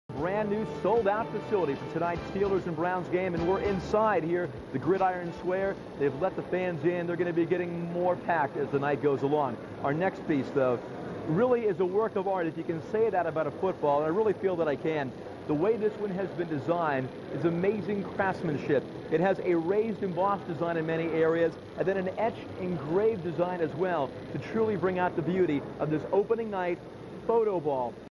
Brian Collard Hosting Cleveland Browns 1st Game Fox Sports Net Live Remote
Brian hosting Cleveland Browns live remote.